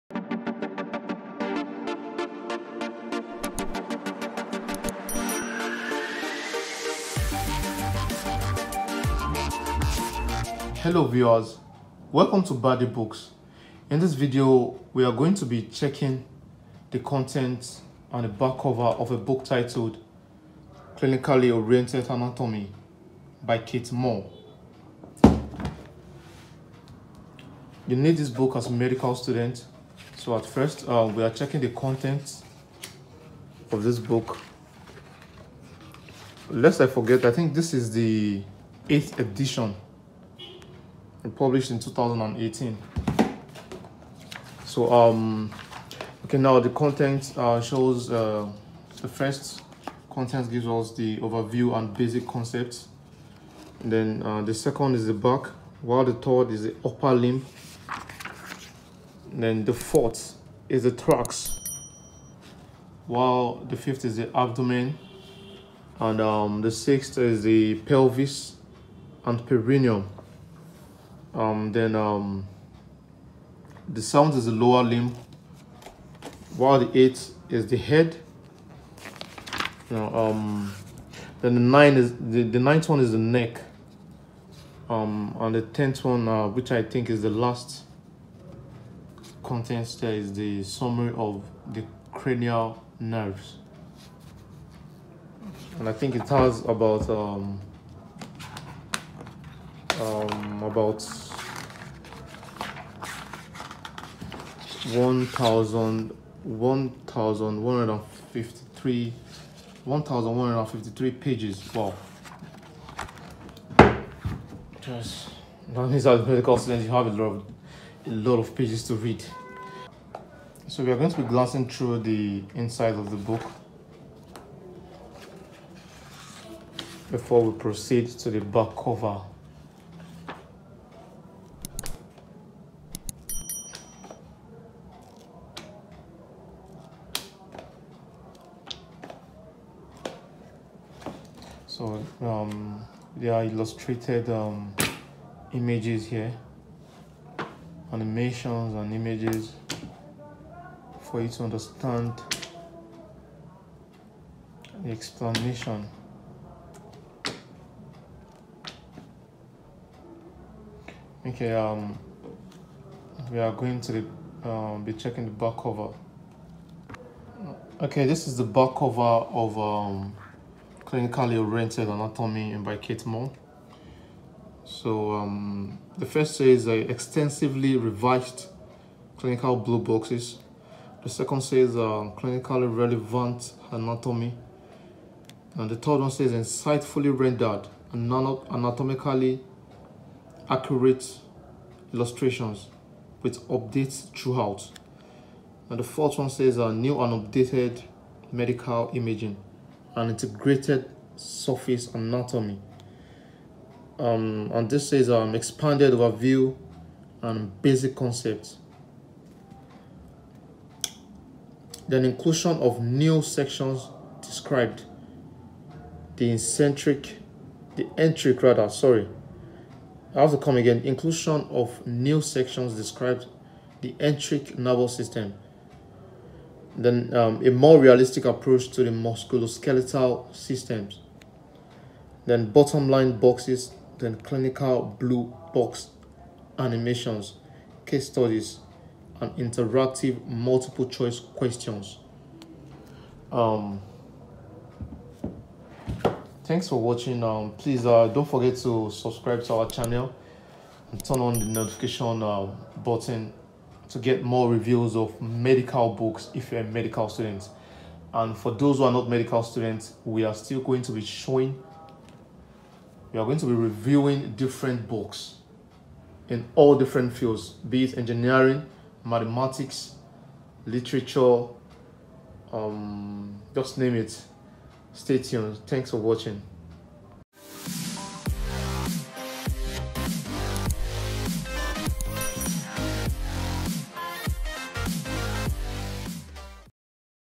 Clinically Oriented Anatomy by Keith L. Moore
Content and back cover check of clinically Oriented anatomy,
FROM THE BACK COVER
The world’s most trusted clinically focused anatomy text
Renowned for comprehensive coverage, the best-selling Clinically Oriented Anatomy guides students from initial anatomy and foundational science courses through clinical training and practice. The eighth edition reflects significant New information and updates and maintains the highest standards for scientific and clinical accuracy. Comprehensive updates reflect changes in the clinical application of anatomy as well as New imaging technologies, focusing on the anatomy that students need to know.
Extensively revised “Clinical Blue Boxes” explore the practical applications of anatomy, and helpful icons distinguish the type of clinical information covered in each.
Clinically relevant anatomy focuses on physical diagnosis, interpretation of diagnostic imaging, and understanding the anatomical basis of emergency medicine and general surgery.
Insightfully rendered, anatomically accurate illustrations, with updates throughout, make this book an aesthetically rewarding learning resource. Combined with many photographs and medical images, these stunning visuals facilitate comprehension of anatomical concepts and retention of “mental images” of anatomical structures.
New and updated medical imaging and integrated surface anatomy within each chapter demonstrates the relationship between anatomy, physical examination, and diagnosis.
Expanded “Overview and Basic Concepts” chapter includes functional systemic anatomy summaries and introduces approaches to studying anatomy, terminology, imaging, and more.
Inclusion of New sections describing the enteric nervous system (considered by some as a “second brain”) and its unique role in the innervation of the digestive system gives students cutting-edge insight into this significant scientific discovery.
A more realistic approach to the musculoskeletal system explores the action and use of muscles and muscle groups in daily activities, emphasizing gait and grip.
“Bottom Line” boxes summarize information to facilitate ongoing review and underscore the big-picture viewpoint.
Clinical Blue Box animations, case studies, and interactive multiple-choice questions facilitate review and self-testing.